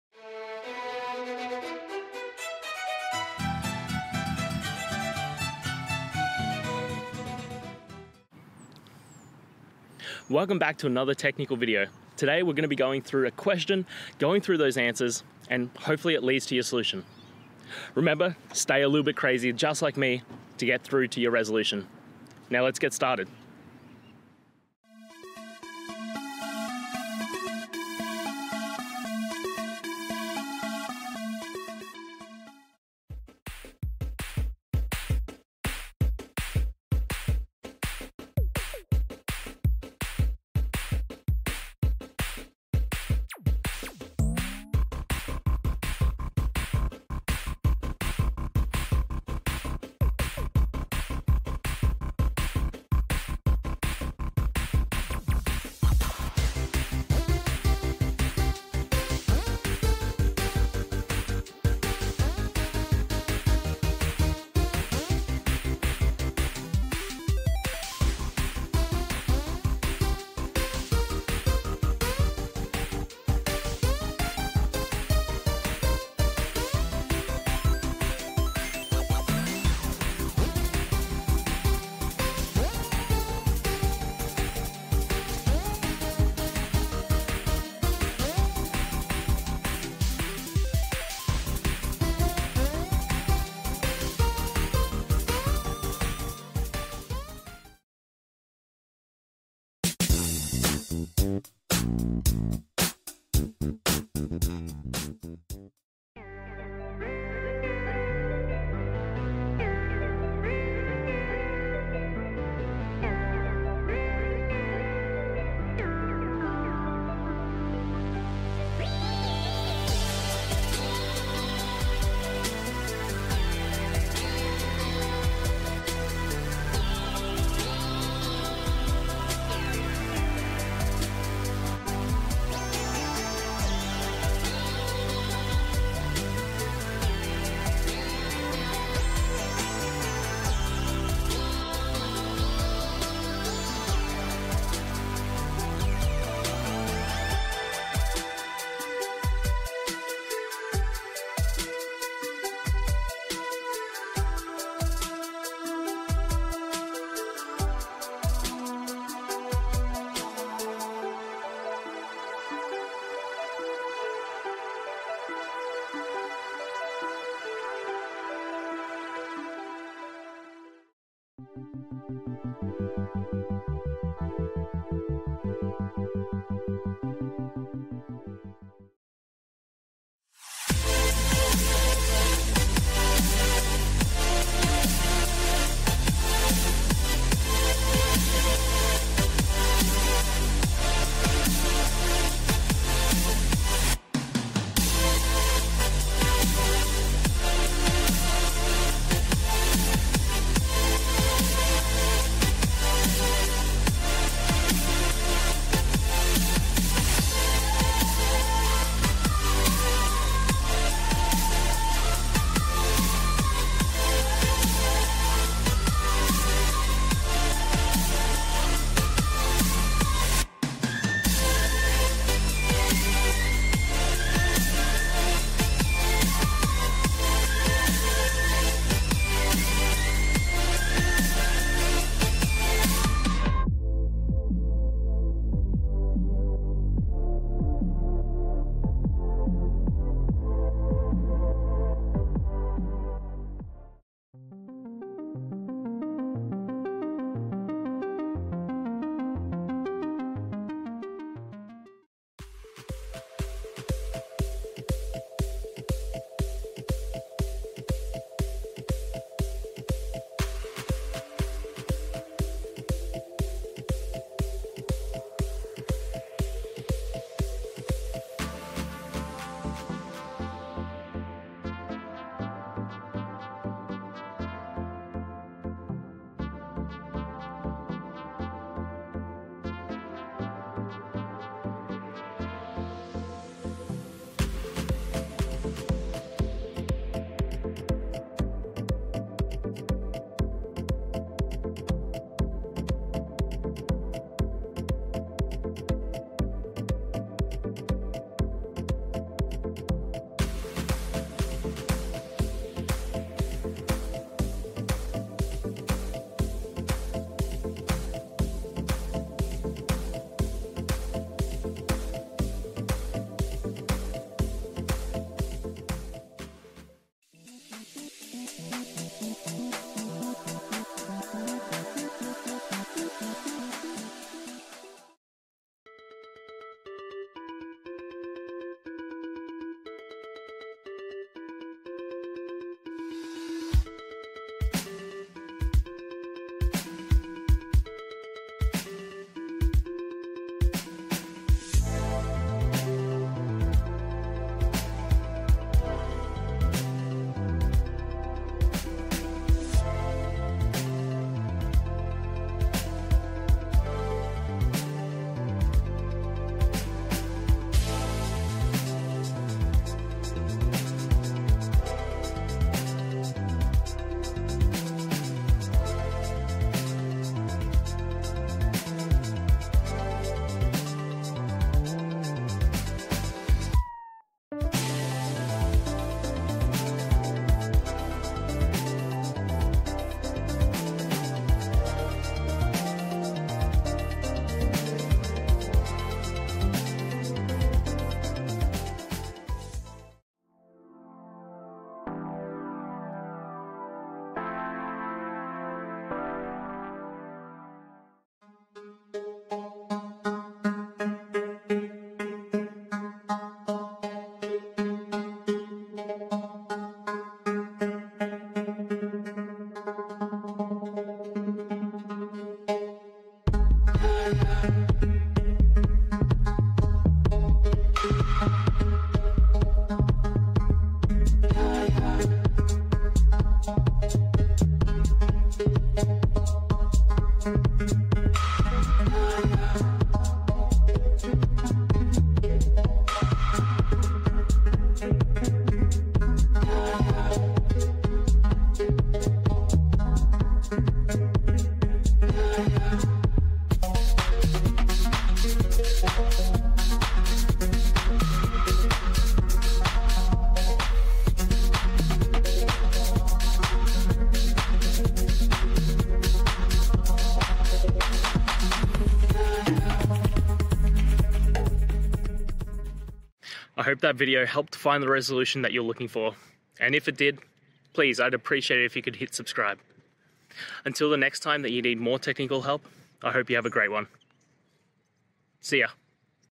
How to Add a Vertical Line Programmatically in an iOS Stack View
In this video, we’ll explore how to enhance your iOS user interface by programmatically adding a vertical line within a stack view. Whether you're looking to improve the visual separation of elements or simply want to add a stylish touch to your app, this tutorial will guide you through the steps needed to implement this feature seamlessly. Join us as we dive into the code and discover the best practices for working with stack views in iOS development!

Today's Topic: How to Add a Vertical Line Programmatically in an iOS Stack View

Related to